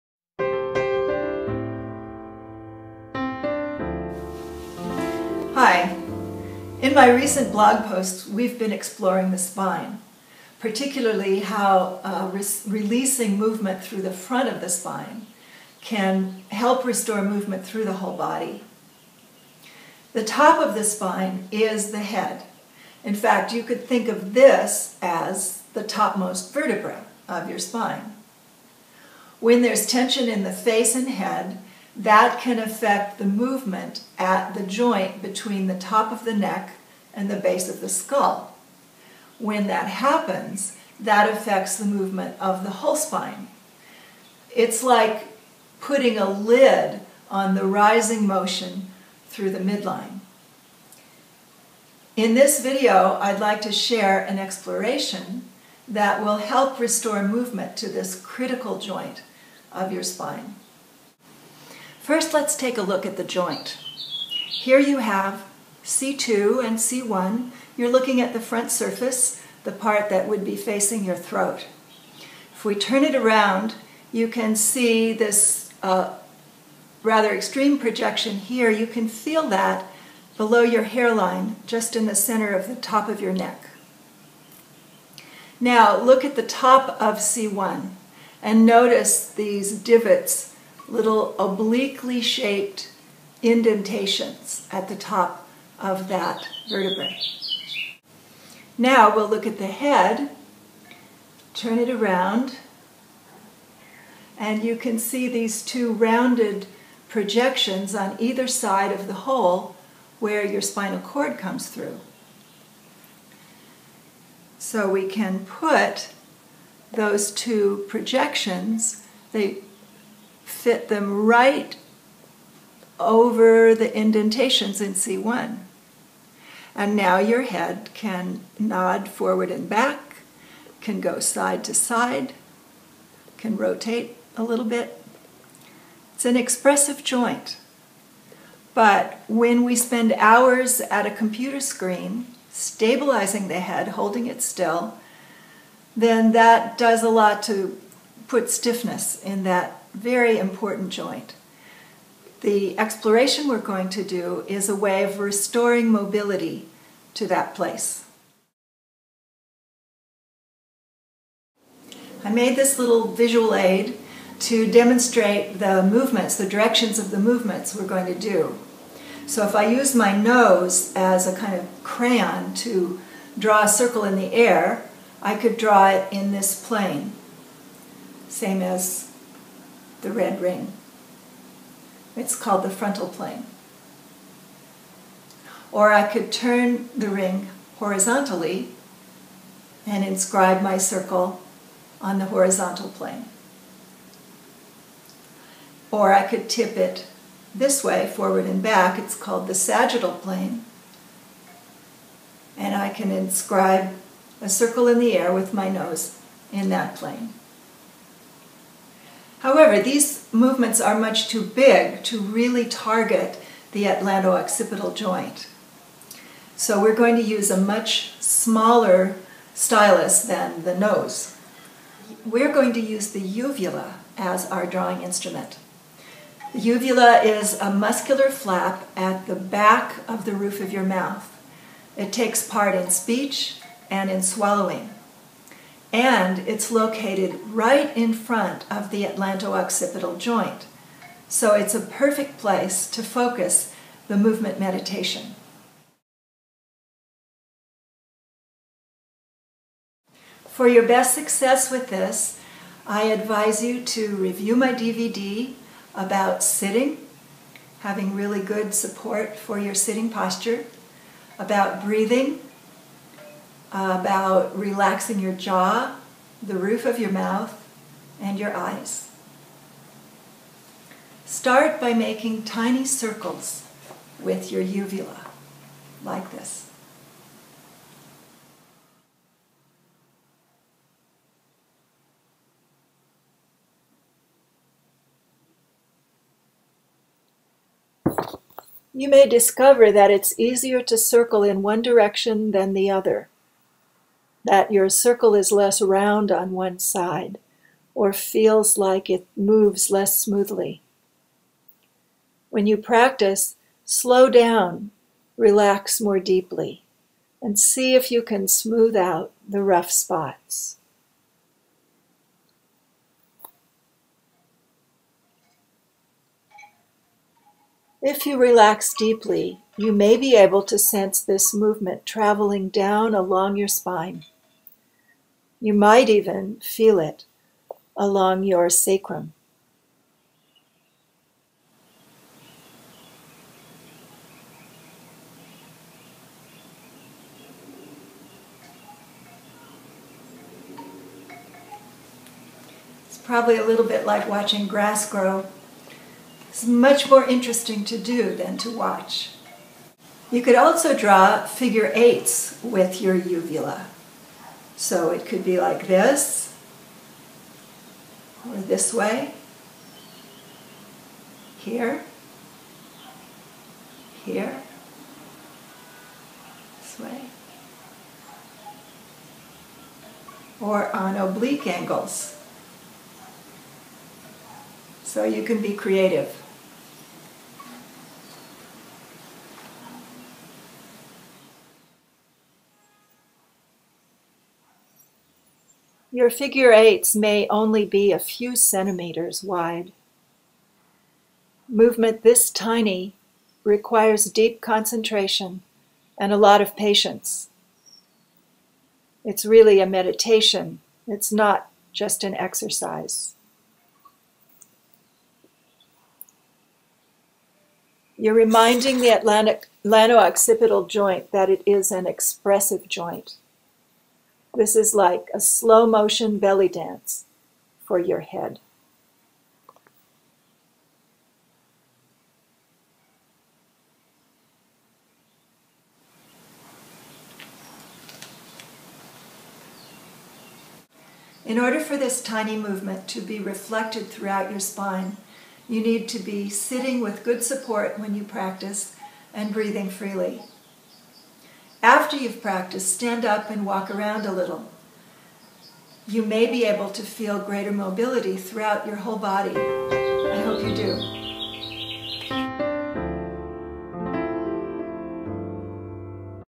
Free Your Head
Mary Bond:  Building Good Posture for Healthy Living
Unlike posture improvement programs that promise instant results, "Heal Your Posture: A 7-Week workshop" with Mary Bond, author of The New Rules of posture, helps you understand the interrelated habits that underlie poor posture.  By spending time working with Mary's suggestions, viewers learn to re-map long-standing habits of walking, standing, sitting and breathing that contribute to unhealthy and unattractive posture, as well as to back, neck jaw and shoulder pain.  Each lesson contains simple anatomy to help you understand your body better, ergonomics tips, and exercises that, once learned, can be incorporated into the activities of daily living.